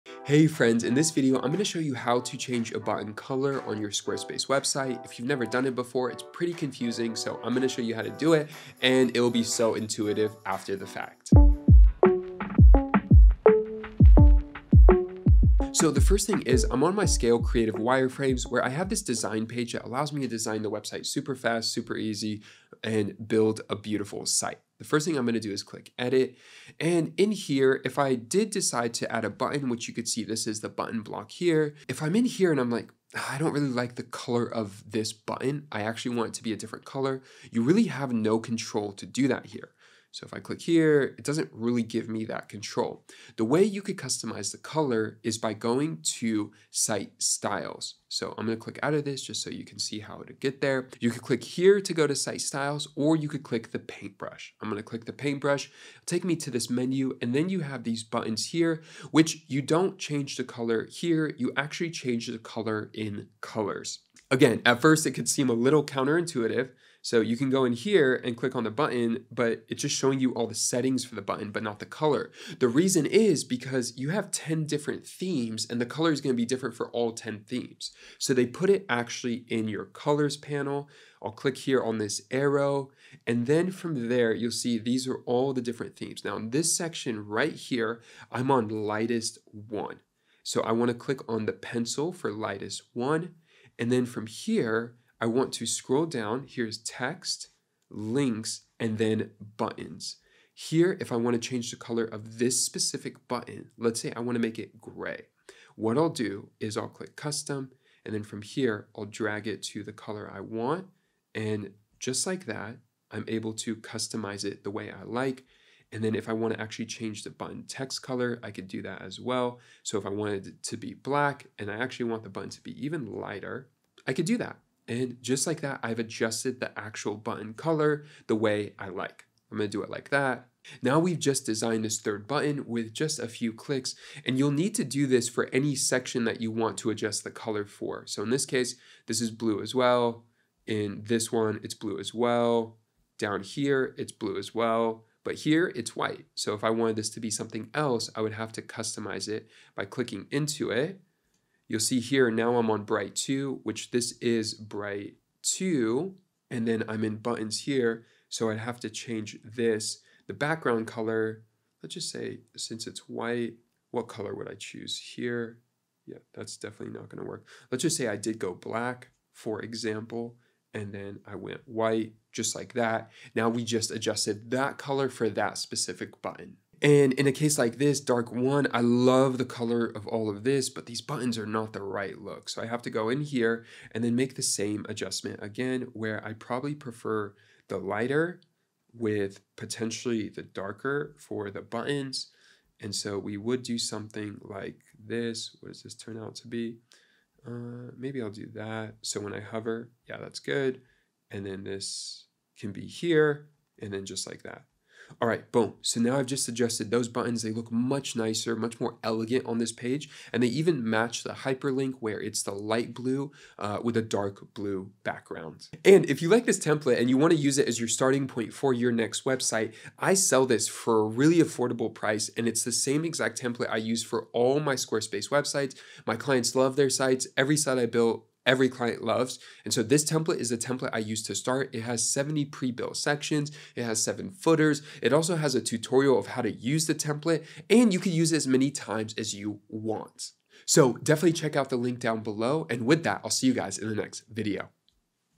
Squarespace How To Change Button Color Styles
🧑‍💻 Save 20% On Squarespace Today! (code ISKANDER20)

In this tutorial, you'll learn how to easily change the button colors on your Squarespace website. The presenter demonstrates the step-by-step process using Scale Creative wireframes, navigating through the site styles and colors panel to customize button colors effectively. By following these instructions, you can make your buttons visually appealing across different sections of your website. Additionally, the presenter offers a customizable template for building elegant Squarespace websites, complete with pre-built sections and footers. Check the link below to purchase and transform your site!

On this channel, we talk about everything related to Squarespace and running an online business.
(code ISKANDER)

00:00 Introduction to Changing Button Colors in Squarespace
00:43 Navigating to Site Styles
01:21 Customizing Button Colors
01:55 Adjusting Button Text Colors
02:10 Applying Changes to Different Sections
02:56 Final Adjustments and Tips
03:35 Template Promotion and Conclusion